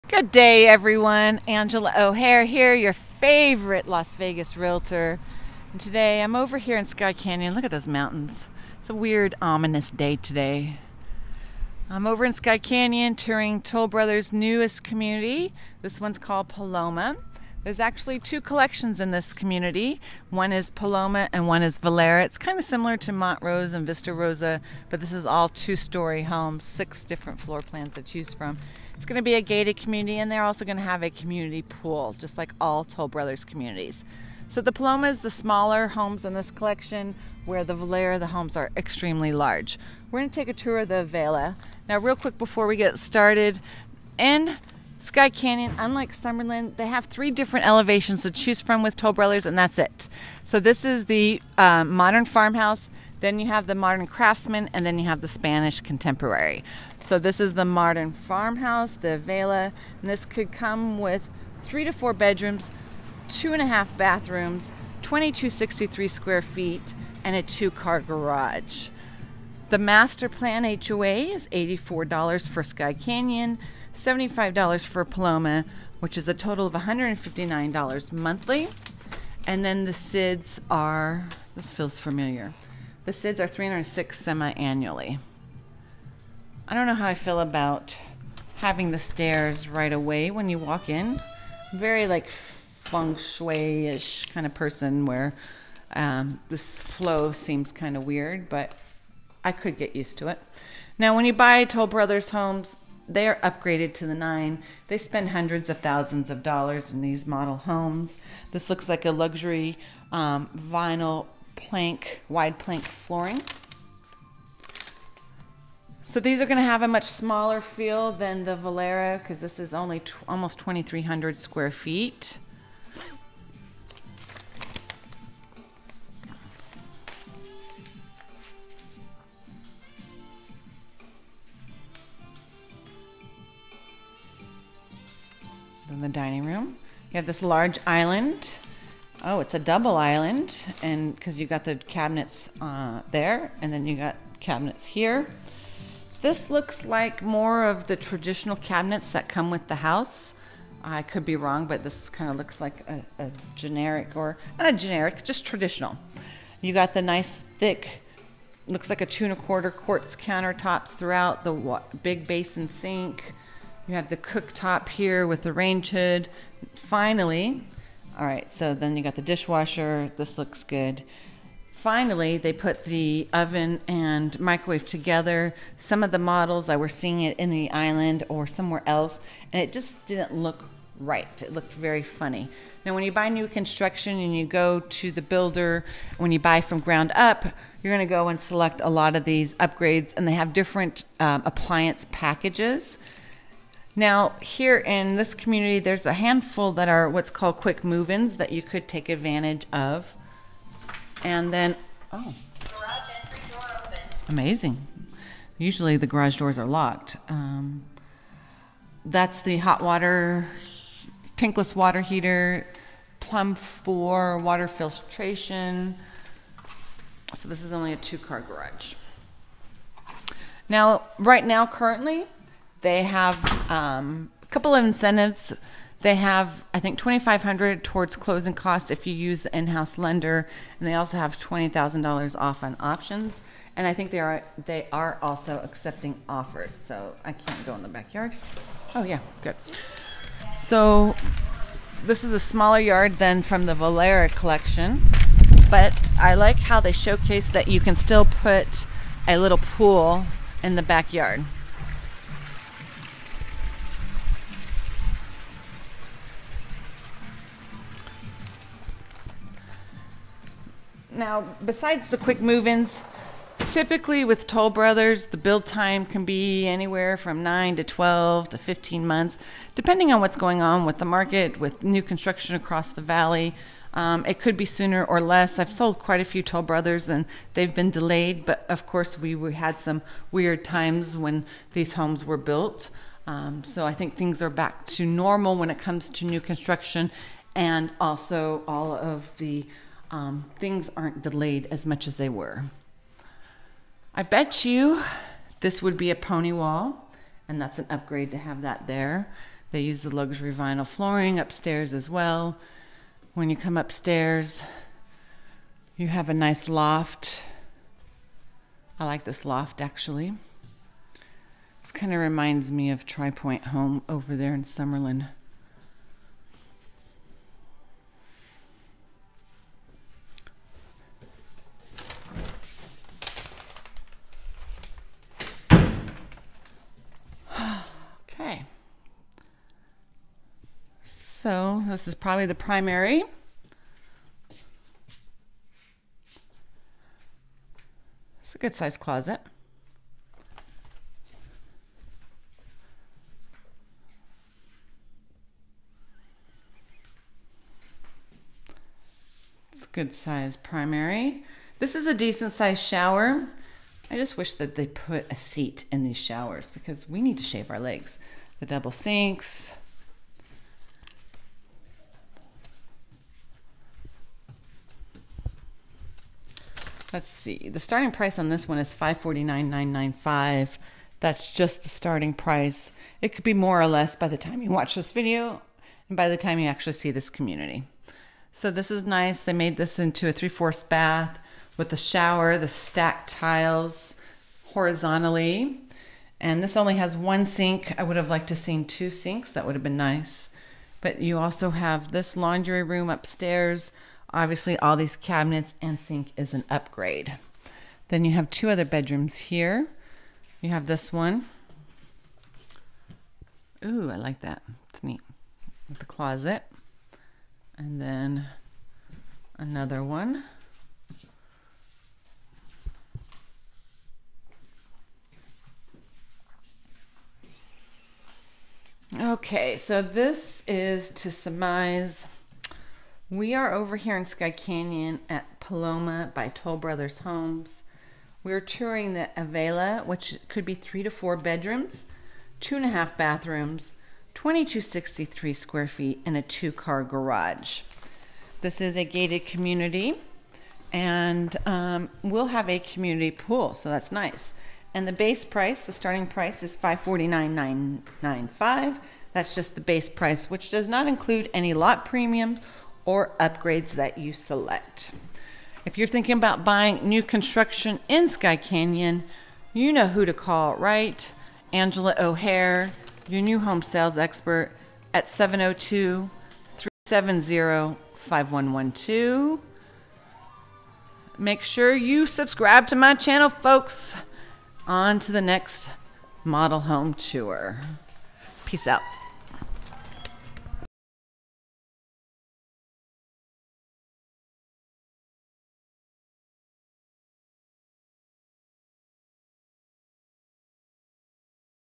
Paloma Collection Toll Brothers in Skye Canyon | Avella Model, $549k+, 3-4 BDS, 2263 sq.ft.
Avella Model Tour - Paloma Collection by Toll Brothers

Join me as a do a model home tour of the Avella Model in Paloma by Toll Brothers in the Skye Canyon Master Planned Community

The Paloma Collection, ideally situated in Skye Canyon, is a new home community offering luxury two-story home designs with flexible living spaces and access to the master-plan amenities. Offering 3 different floor plans to choose from ranging in size from 2,263 sq. ft. to 2,543 sq. ft. with 3-5 bedrooms, 2.5-3 bathrooms and 2 car-garages.

PALOMA
2,263 sq. ft.
3-4 beds
2.5 baths
2 car-garage

HOA FEES
$50 a month - Paloma
$84 a month - Skye Canyon
SID $306 Semi-Annually

Company: Home Realty Center
License #: 180246